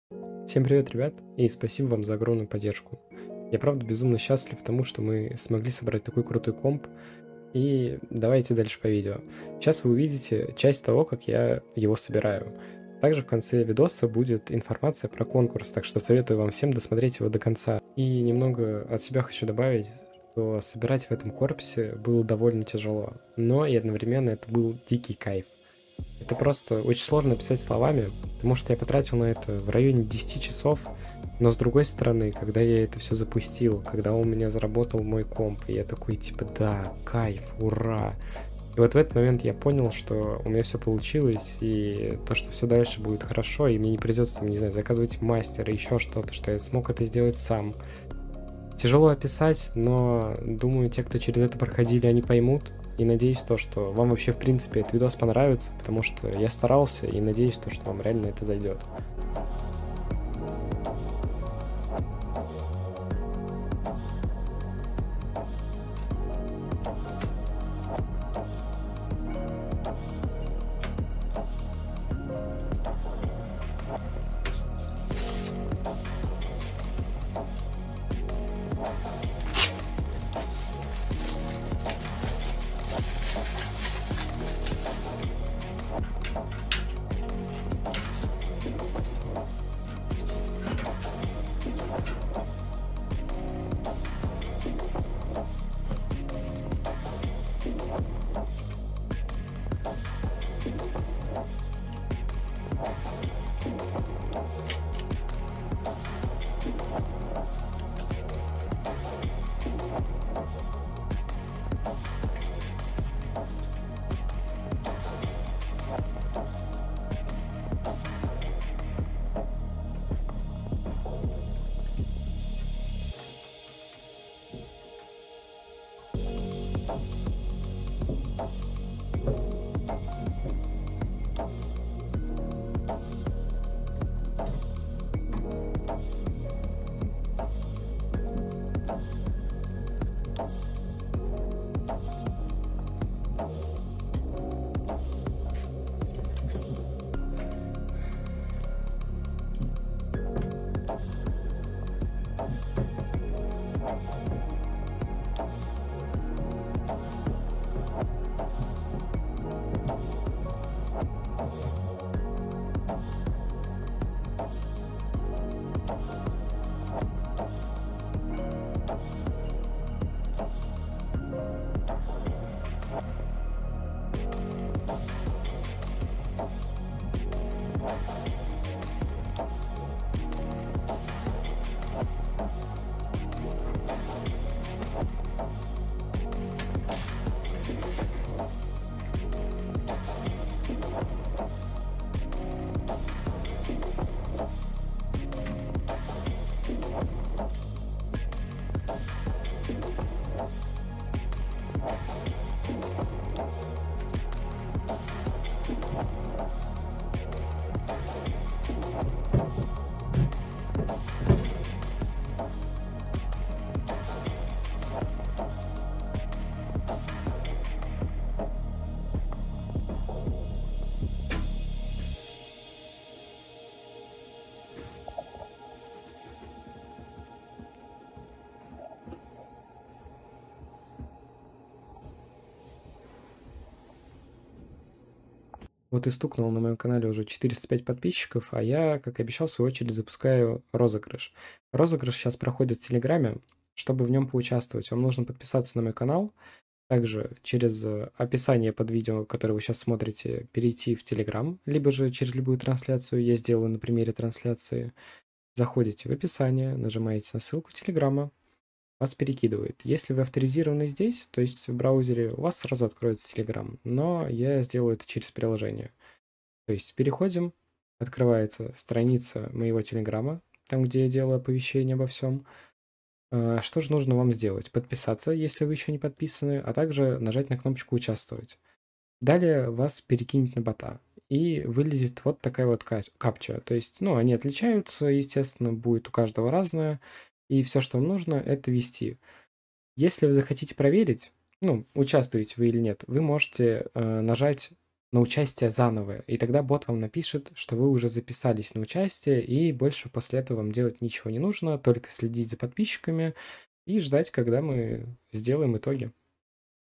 Подарили игровой компьютер 🎁 *Анонс розыгрыша! 🎁
Мой пк:
Видеокарта Colorful 3060 12gb
Процессор Intel Core i5-12600K OEM
Материнская плата MSI MAG B660M MORTAR WIFI DDR4
Блок питания Cougar GEX850
Озу: XPG 3200MHz 32GB
Корпус Lian Li LANCOOL III
Кабель менеджмент конечно у меня пока не супер,но я не техно-блогер,так что займусь я им позже)